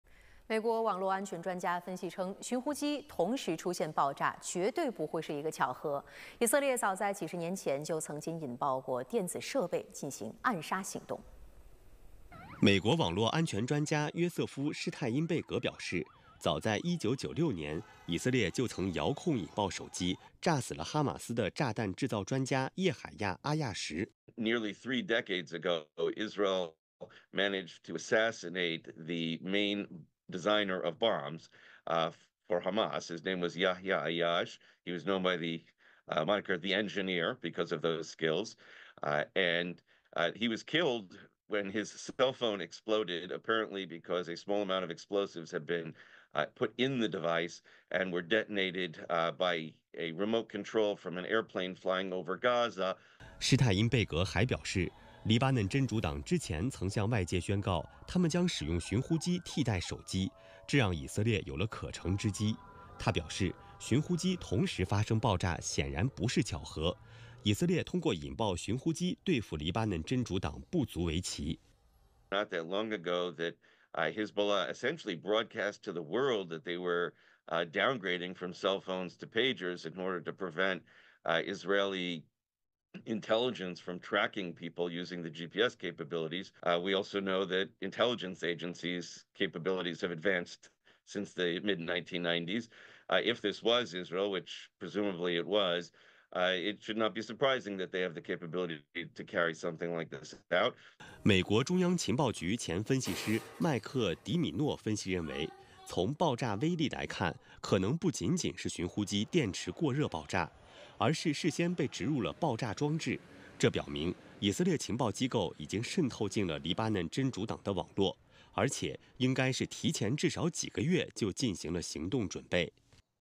[24小时]黎巴嫩多地发生寻呼机爆炸事件 安全专家：以数十年前就有遥控引爆能力|新闻来了 News Daily
黎巴嫩多地发生寻呼机爆炸事件·安全专家：以数十年前就有遥控引爆能力。